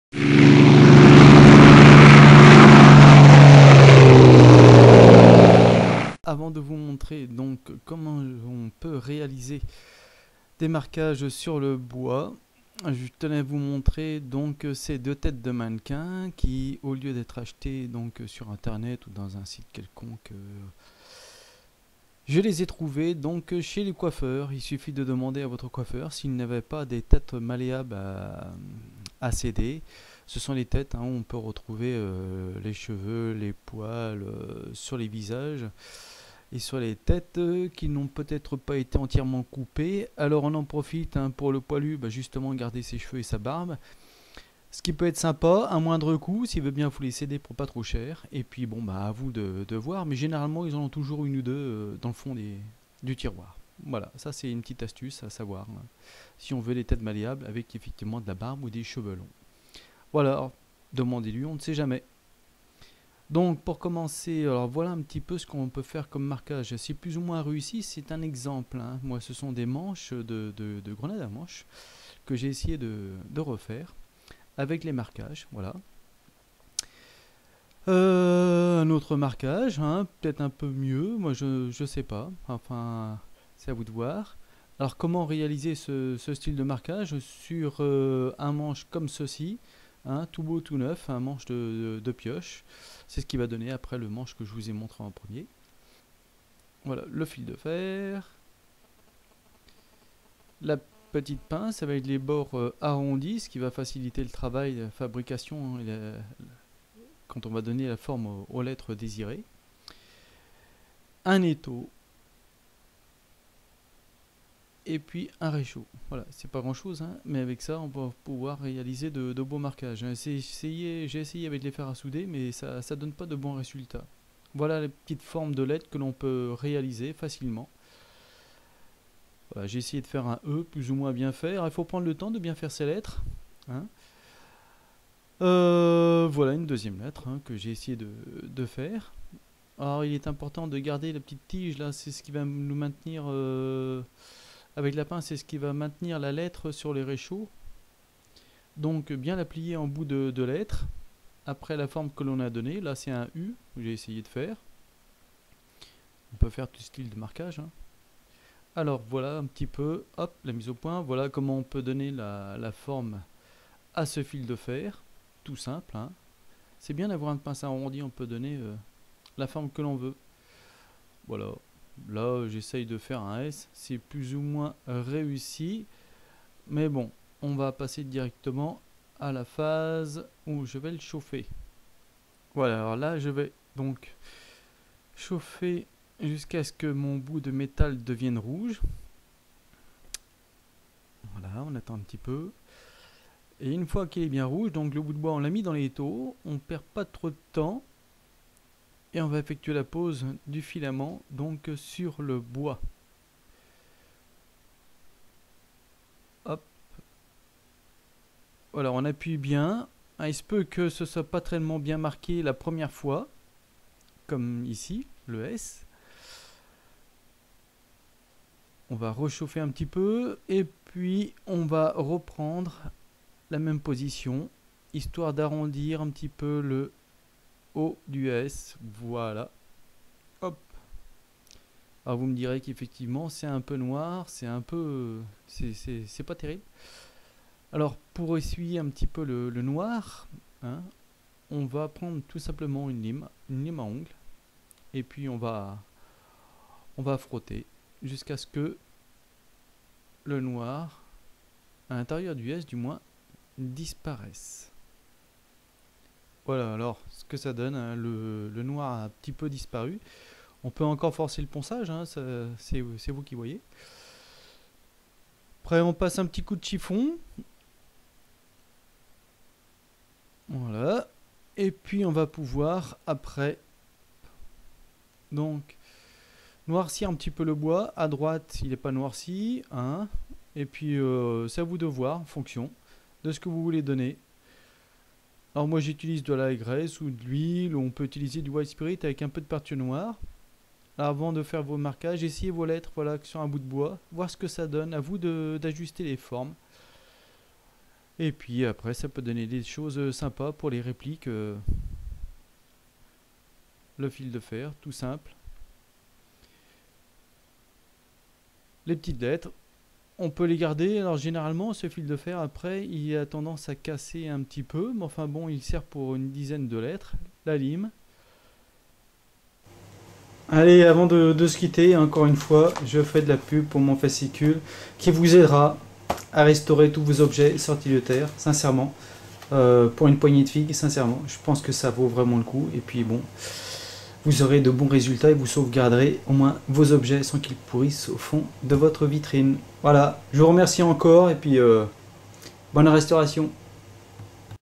comment effectuer un marquage sur bois
Une solution simple pour effectuer un marquage sur le bois. J'ai essayé pas mal de choses, mais celle-ci me semble être la meilleur.